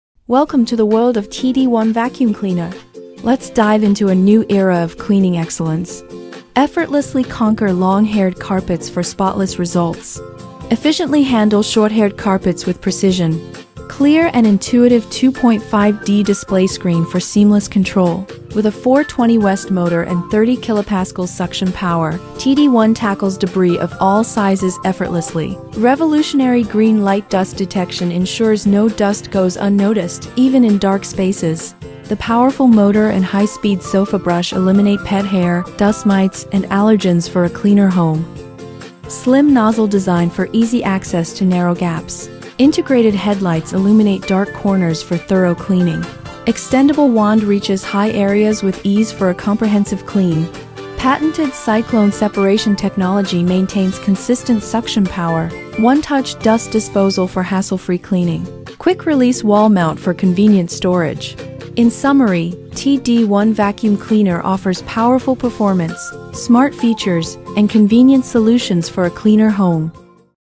TD1 Vacuum Cleaner: The Ultimate Solution for Easy Cleaning
TD1 Vacuum Cleaner, the ultimate solution for all your cleaning needs! With powerful suction and an efficient filtration system, it easily removes dirt and dust of all kinds. What's more, TD1 is equipped with intelligent green light dust detection technology, allowing you to see hidden dust and dirt in dark corners. In addition, TD1 features a user-friendly design and portability, enabling you to clean anytime, anywhere. If you're looking for a high-performance, intelligent, and easy-to-use vacuum cleaner, TD1 is definitely your best choice. Check out this video to learn more about its powerful features!